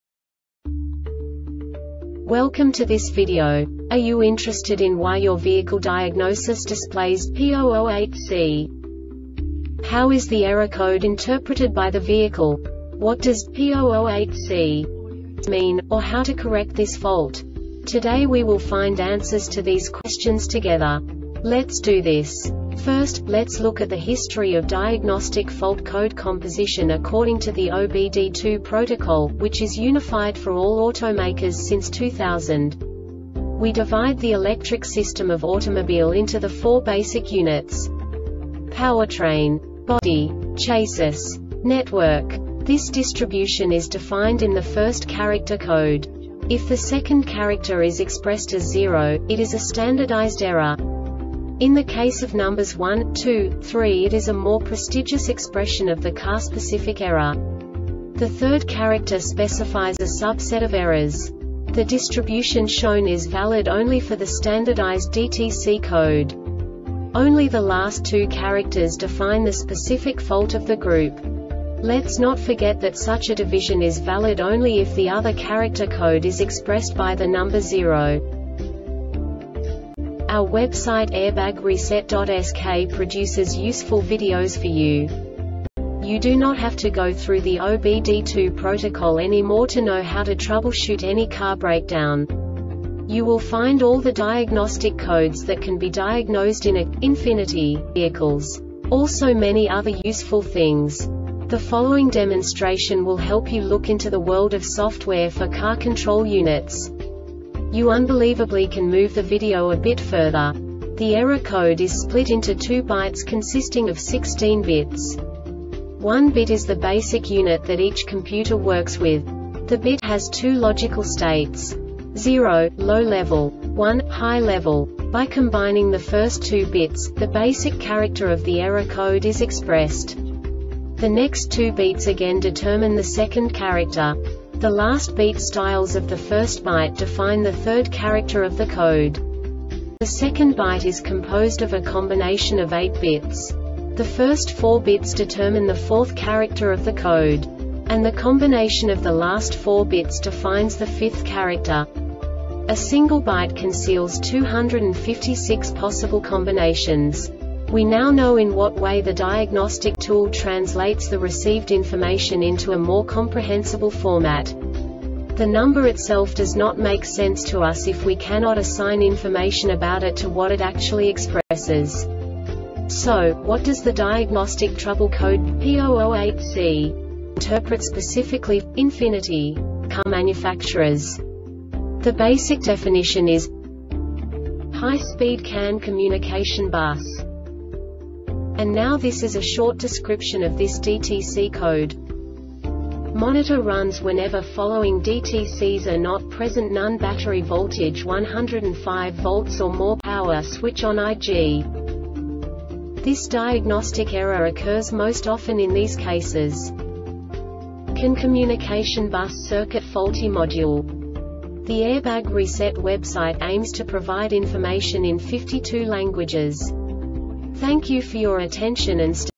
DTC Ford P008C Short Explanation
Contents:
0:21 Basic DTC analysis according to OBD2 protocol standard.
1:48 Insight into programming
2:58 A diagnostically understandable description of the Diagnostic Trouble Code
3:05 Basic error definition

Make:
Ford
Code:
P008C
Definition:
High Speed CAN Communication Bus
Description: